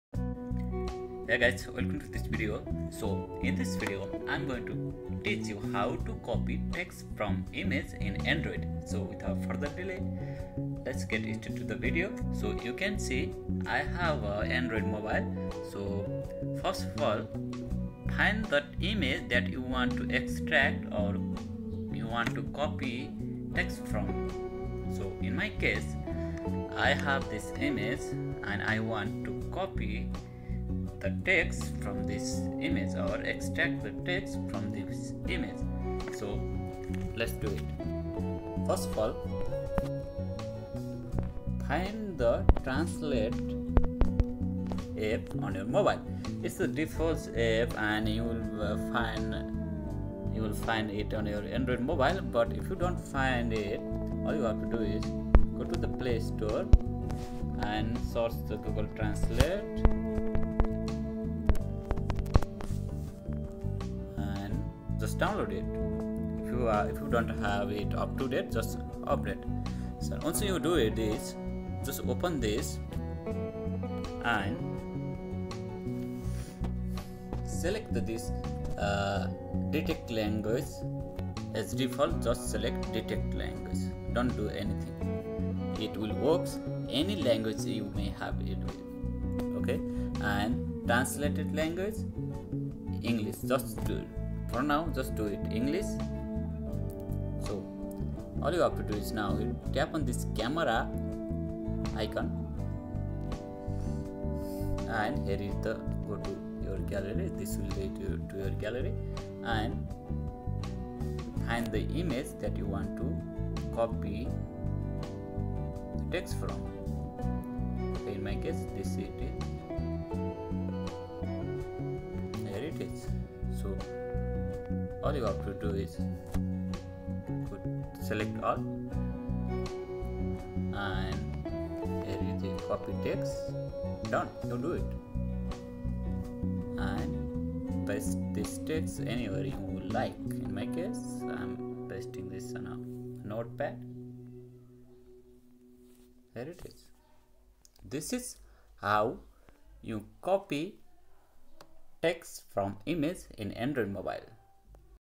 How to copy text from image in android
In this video, I'll show you how to copy text from an image in Android. This tutorial is useful if you need to copy text from an image for a project or homework.

If you're ever in a situation where you need to copy text from an image, this video will teach you how to do it quickly and easily. We'll use Android's built-in image editor to copy the text from the image! This is a useful technique for copying text from an image for a project or homework